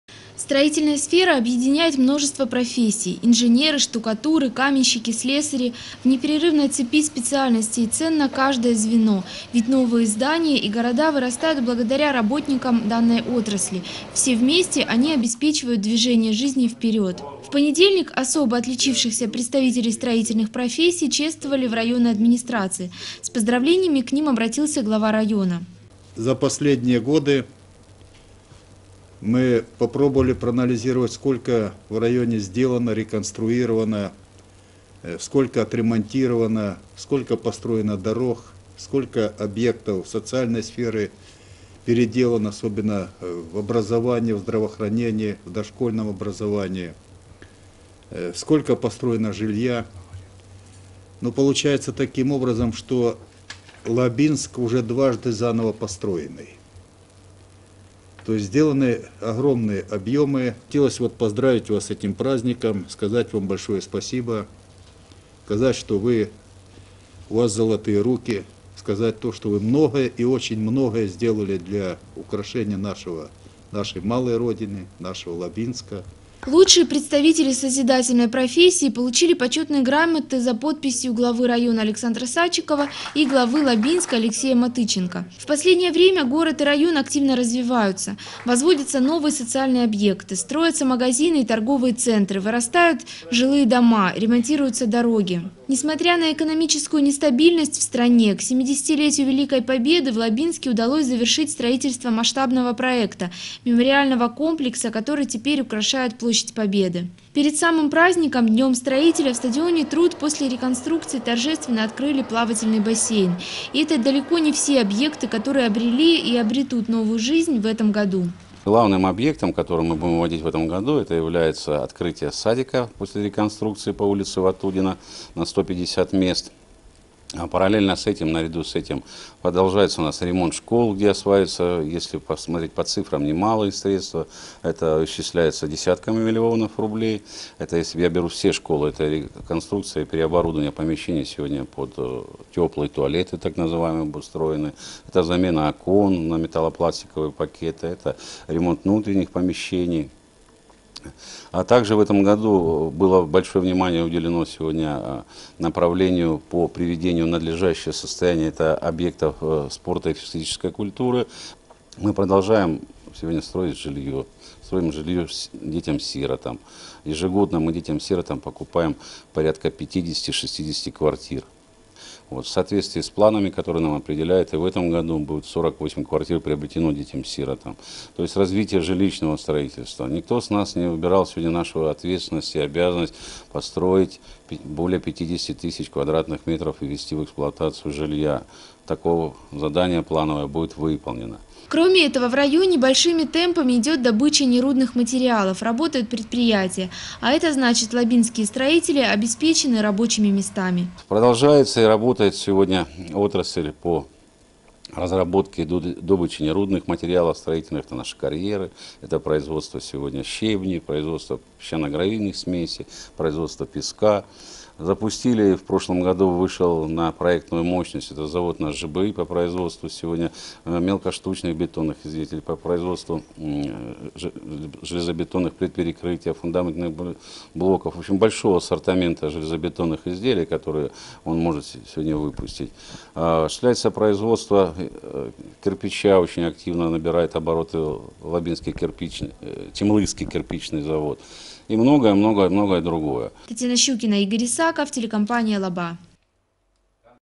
День строителя.
Лучших специалистов строительных профессий отметили и наградили. Чествование прошло в администрации района.